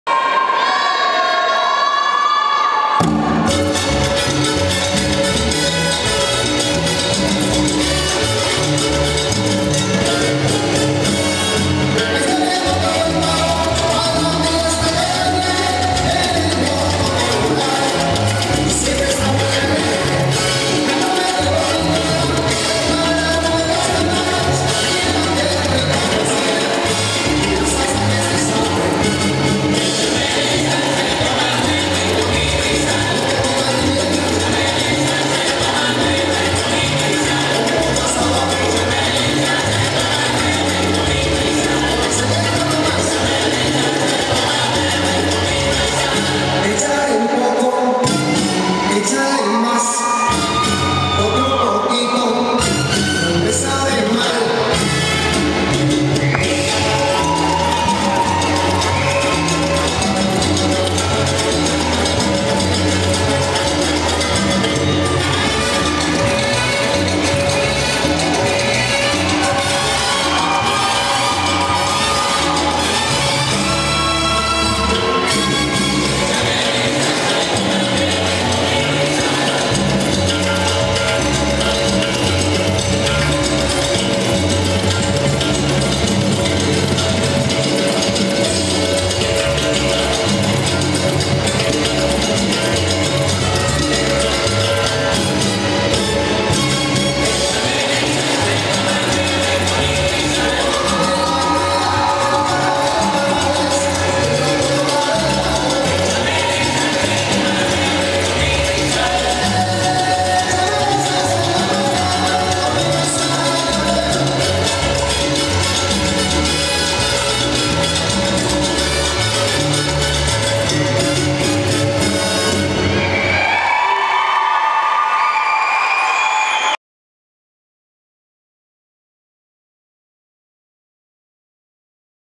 TDSF 2018, Yetişkinler, B Klasman, On1, Final, Show, Cengiz Kağan Hakan, Güler Ezgi Özel, Tarz Dans
Fitdans Organazitörlüğünde düzenlenen
TDSF 2018 Kulüpler arası sportif salsa yarışması
Yetişkinler  B Klasman On1 Final Show
Cengiz Kağan  Hakan & Güler Ezgi Özel
Tarz Dans Gençlik ve Spor Kulübü